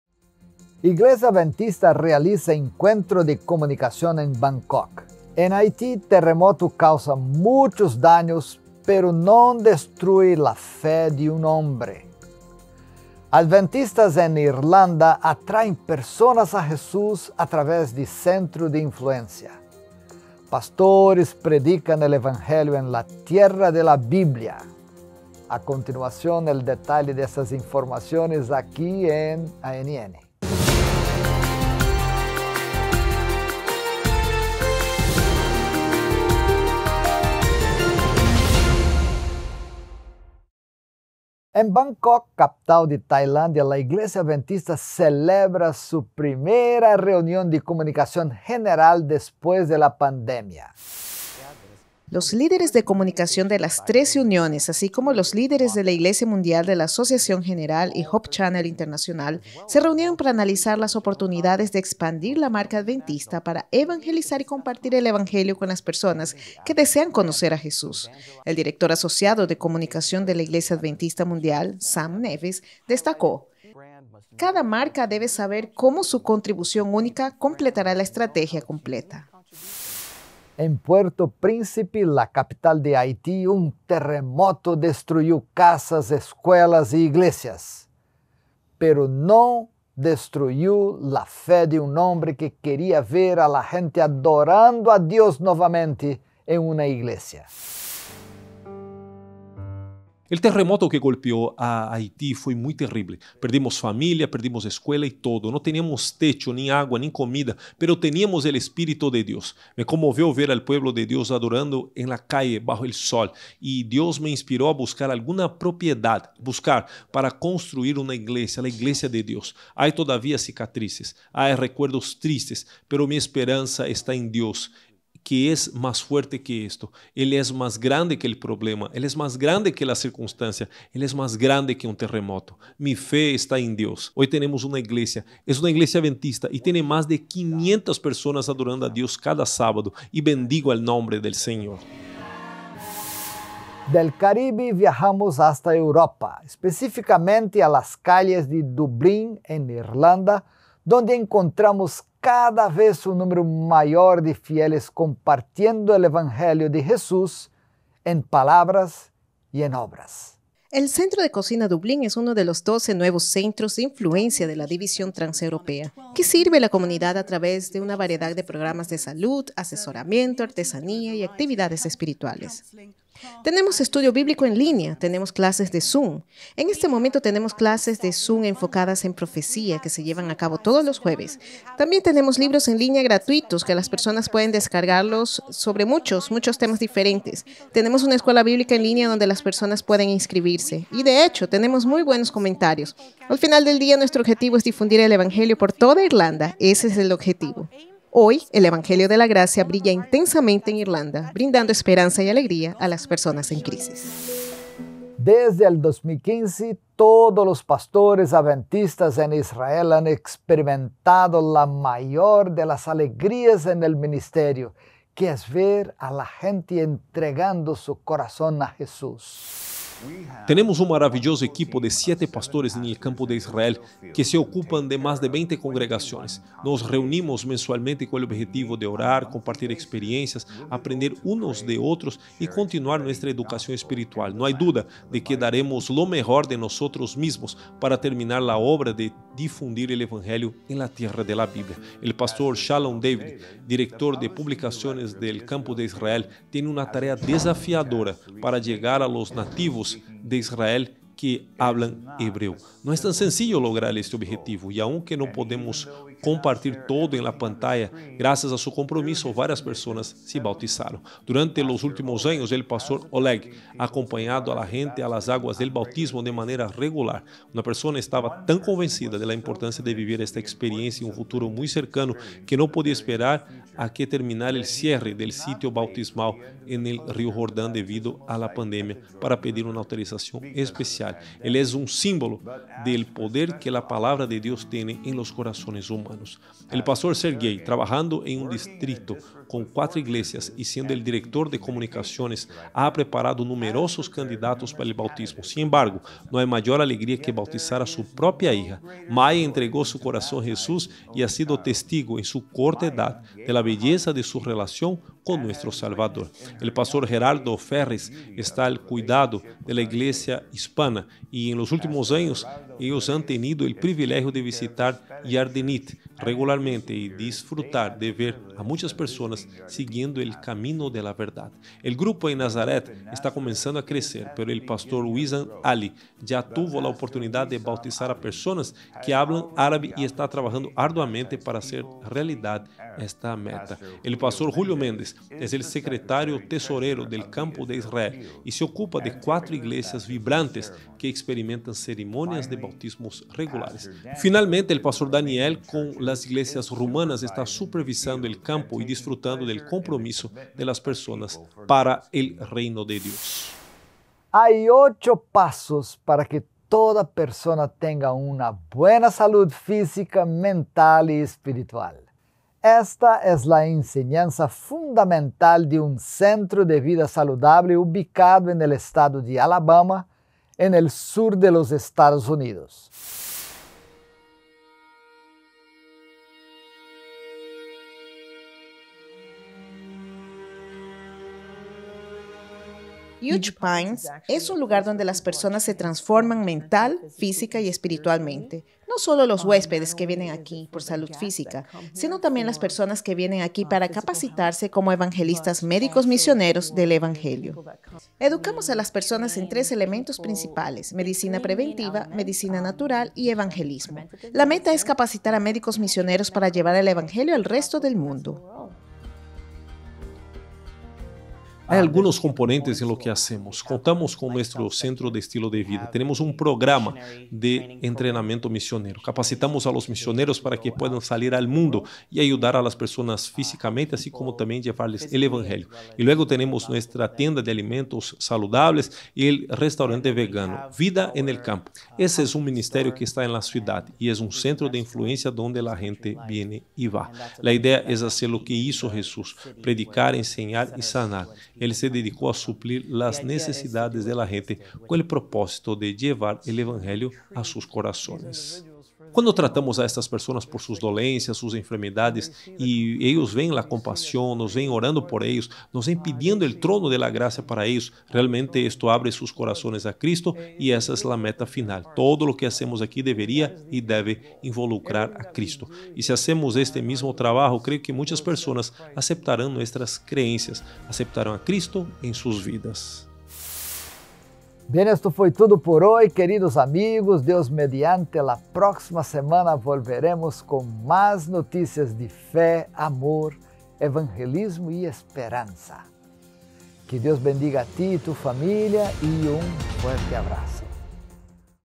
En Haití, terremoto causa daños, pero no destruye la fe de un hombre |Notícias Mundiales Adventistas
Hombre reconstruyó iglesia y hoy hace proyectos evangelísticos.